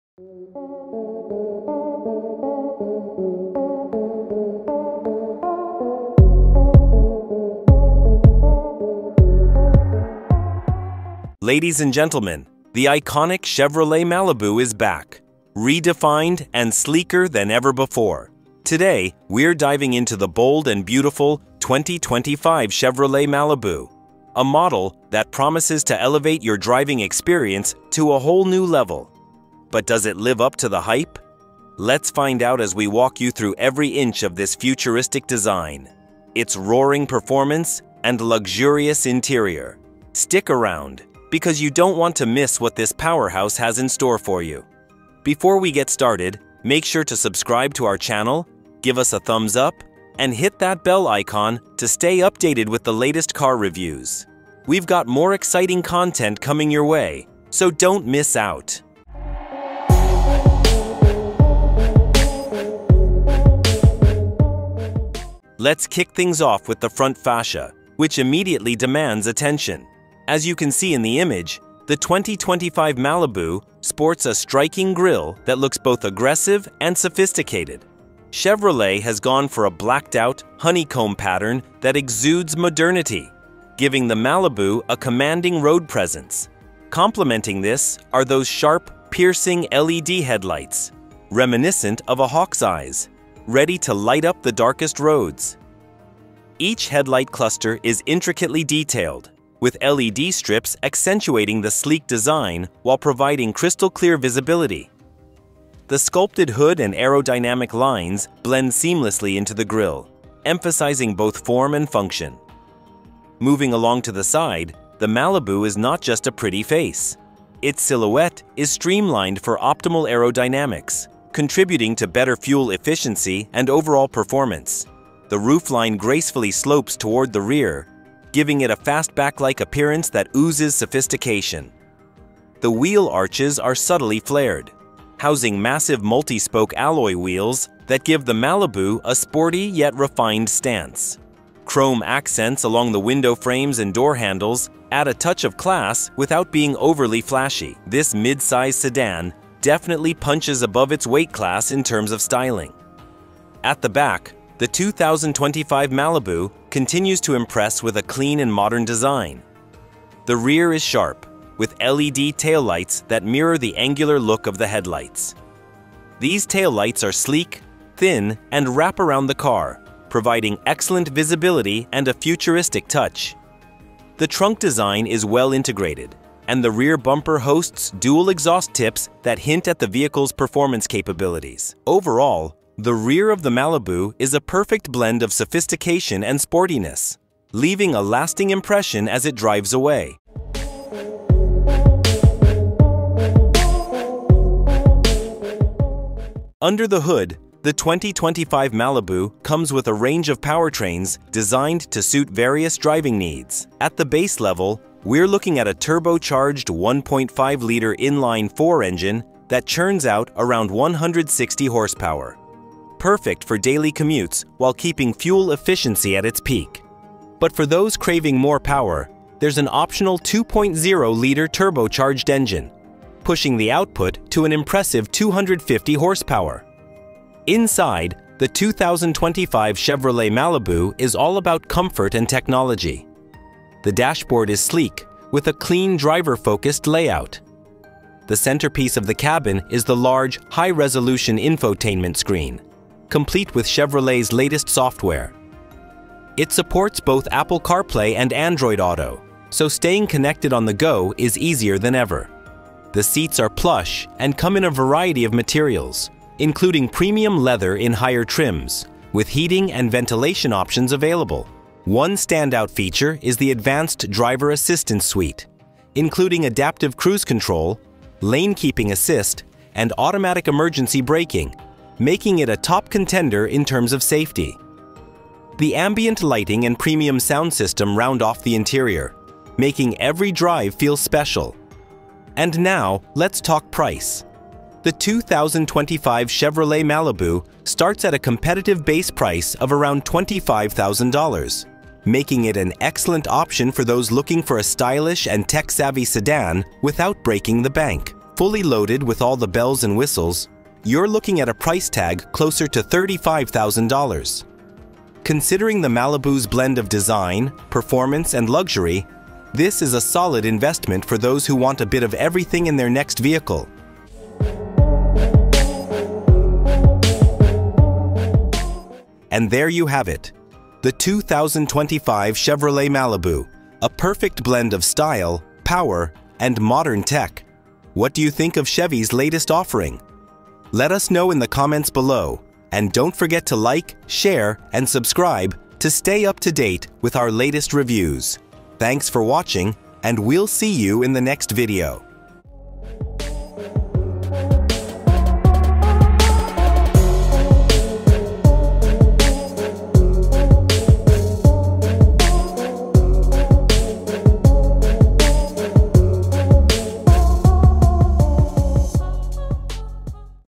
2025 Chevy Malibu is Finally Here - Is This the Family Sedan You Need?
2025 Chevrolet Malibu - Get ready to be amazed by the all-new 2025 Chevrolet Malibu! In this video, we take you on an in-depth tour of Chevy’s latest family sedan, showcasing its stunning design, powerful engine options, cutting-edge technology, and luxurious interior. From its bold front grille to the sleek, modern rear, the Malibu is not just built for comfort—it’s designed to impress. Whether you're looking for performance, safety, or style, this car has it all. Watch the full review to see why the 2025 Malibu is the ultimate family sedan that will leave you speechless!

Next-GenCar